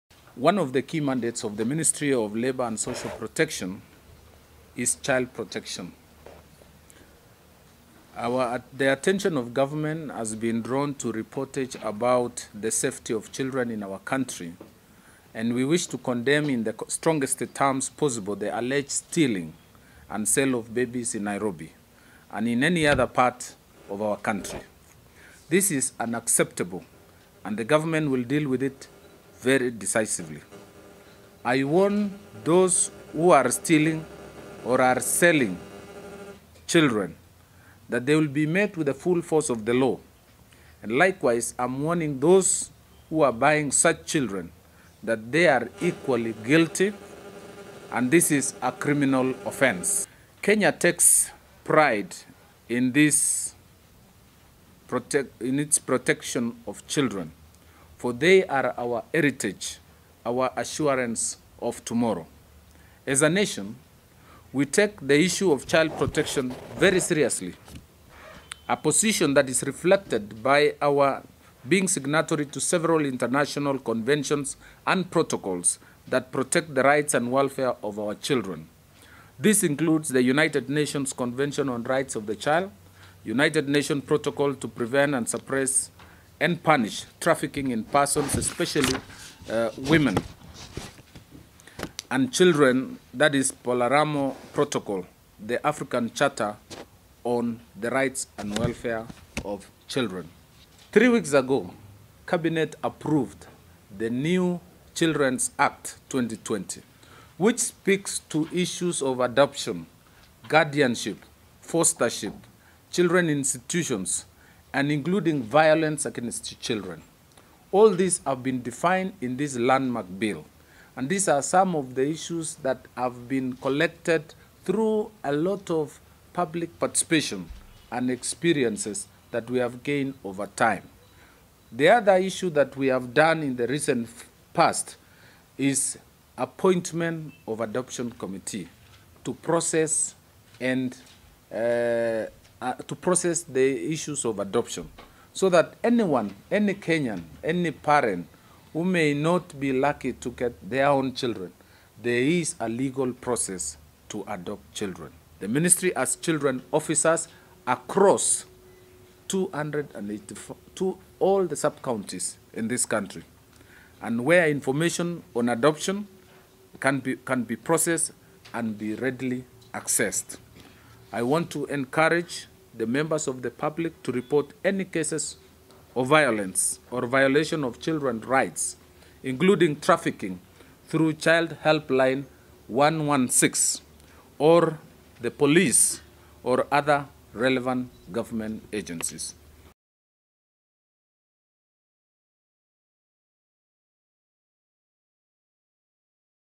GOV'T LAUNCH INVESTIGATION OVER NAIROBI BABY BUYING BBC EXPOSE
The government through the ministry of Labour and social protection headed by CS Simon Chelugui has defended itself from failing to protect children rights after an expose by various media houses on child trafficking in Nairobi, The CS has said that the government takes serious issue of child protection through various acts including Cabinet approving the new children act 2020, at the same time the CS has assured the country that anybody found guilty of such action will face the full force of the law and has urged Kenyans to report any case of child violation identified.